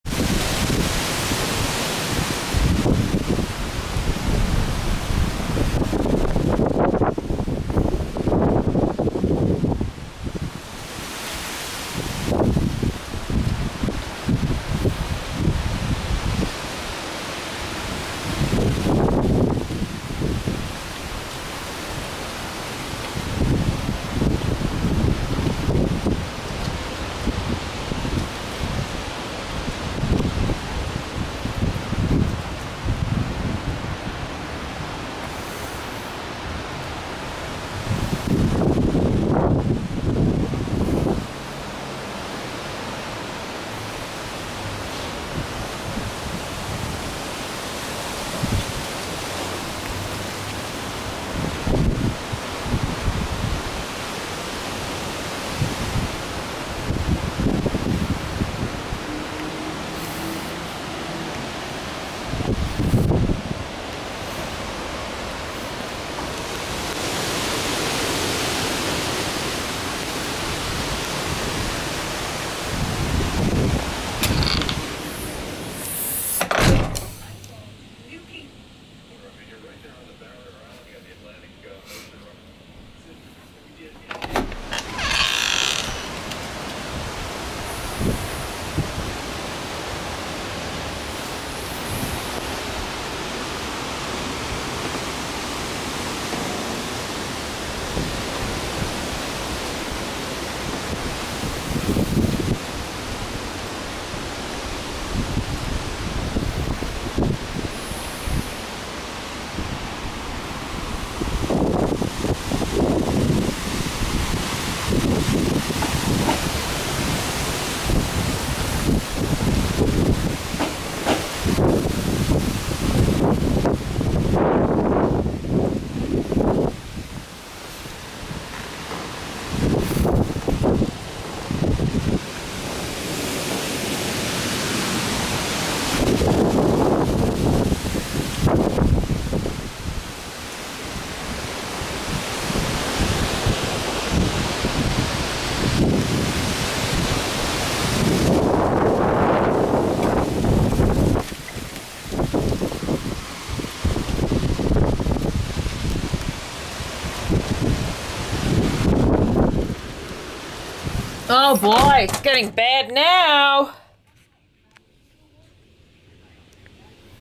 WindsInMyBackyard10292012
Winds in my backyard earlier today. Hurricane Sandy hasn't even arrived here yet the winds are strong enough to move things around. The full moon is going to increase gravitational forces creating even more severe weather.  The eye of the storm will be arriving here in about 4 hours or so. I hope our big trees and my pergola holds up. The central part coming around in Atlantic City soon but will hit us in about 4 hours.